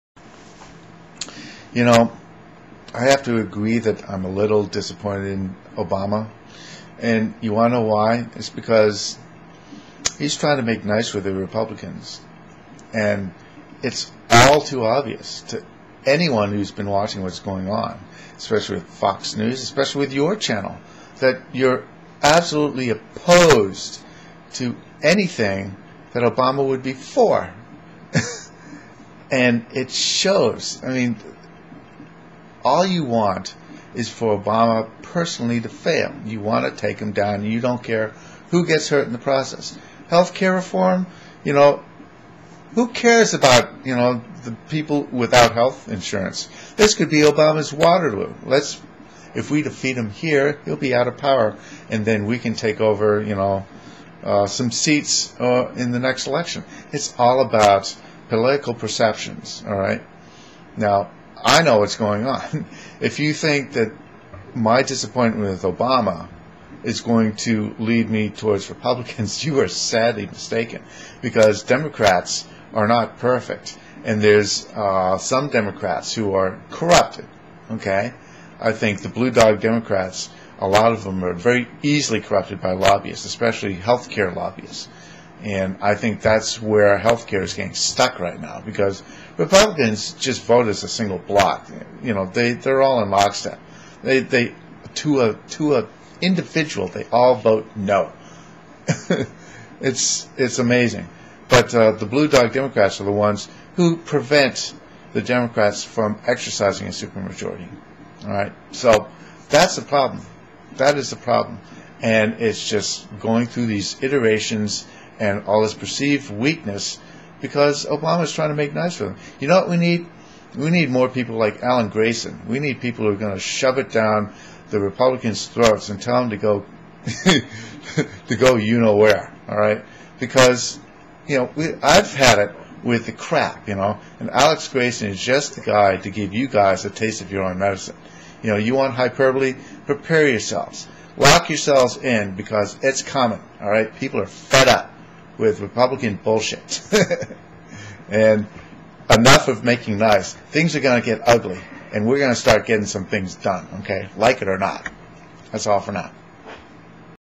Disappointment is Obama does not translate into support for Republicans
Obama is still trying to play nice. Brace yourself for more Alan Grayson and and others who are fed up with Republicans.